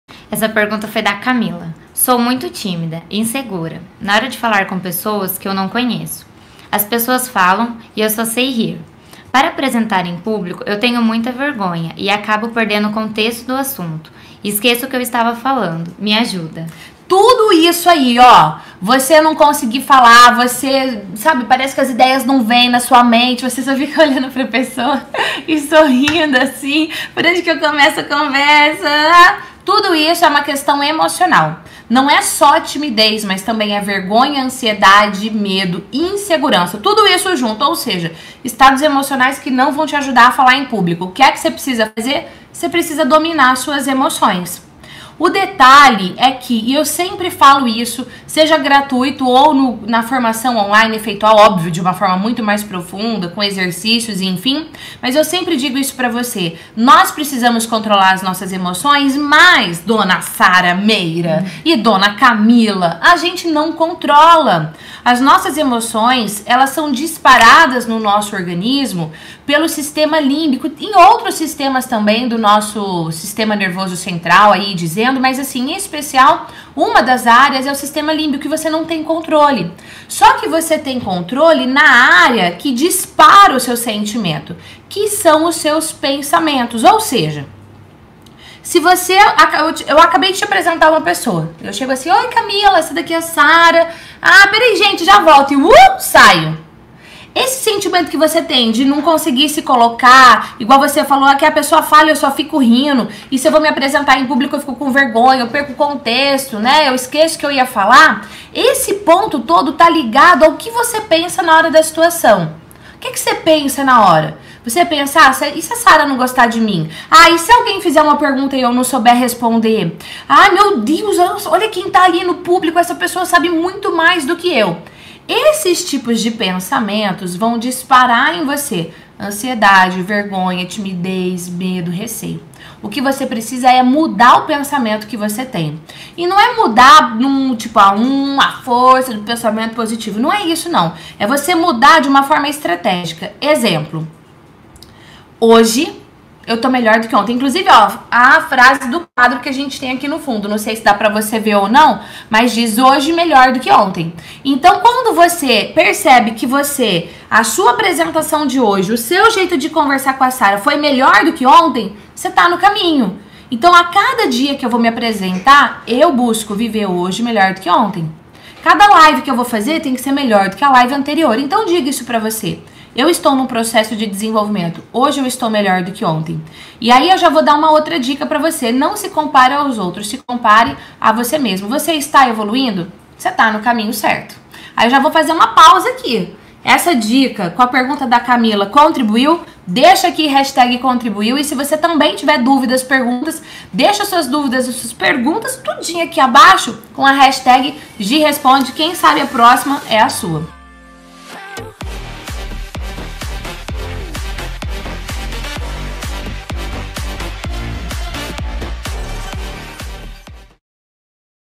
Timidez e Insegurança - Como lidar com a timidez e a insegurança?
Timidez e Insegurança - Como lidar com a timidez e a insegurança? Você conhece alguém que já tentou perder a timidez, ou vencer a timidez? Você já se perguntou como perder a timidez ou como vencer a timidez? No vídeo de hoje eu conto como superar a timidez, dicas para que você acabe com a timidez e a fobia social. Mais que isso, eu falo sobre auto estima, e como melhorando a autoestima você pode aprender como vencer a insegurança, como lidar com insegurança e como tratar a ansiedade. Gostou desse tema? Me conta nos comentários!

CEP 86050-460